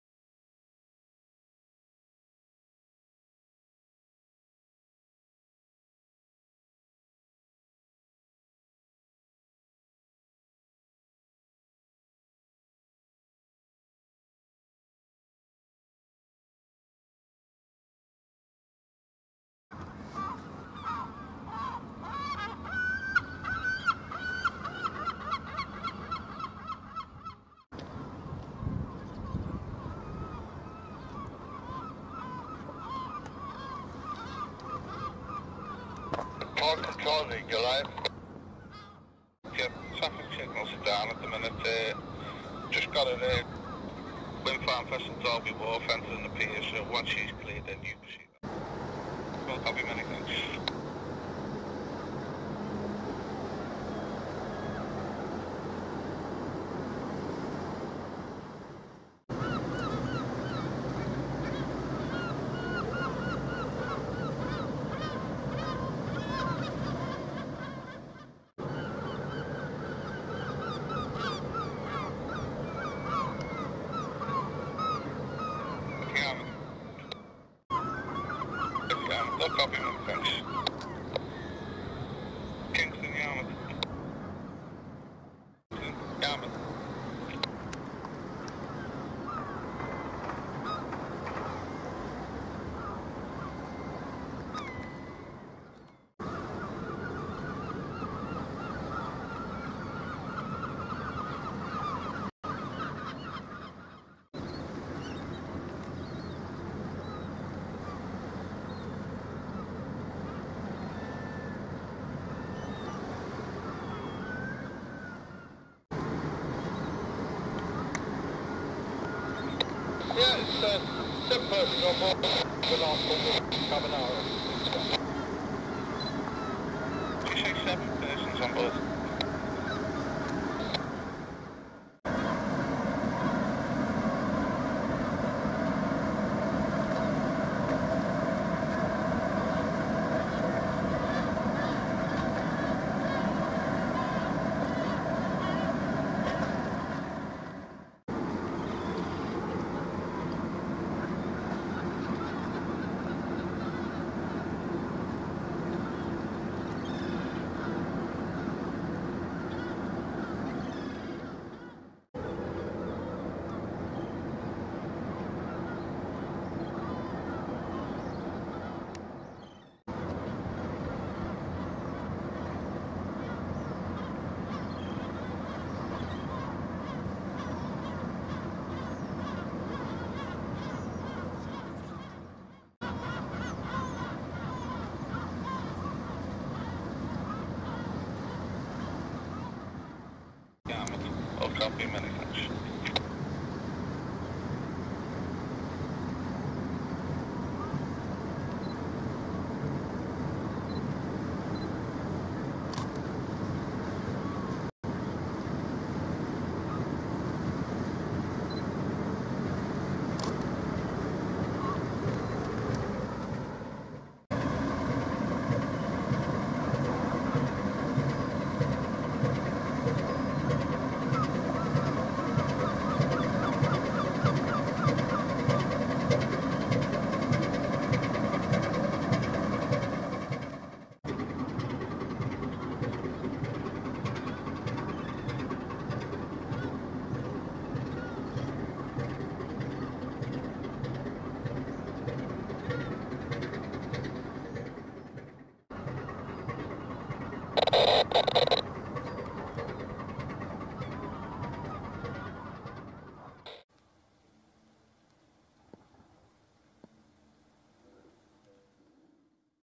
Tugs GOLIATH and KINGSTON depart Great Yarmouth 2/8/18
After bringing general cargo vessel kine into the river port of great yarmouth the 2 tugs goliath and kingston quickly turn round and head back out of the river and head back for lowestoft.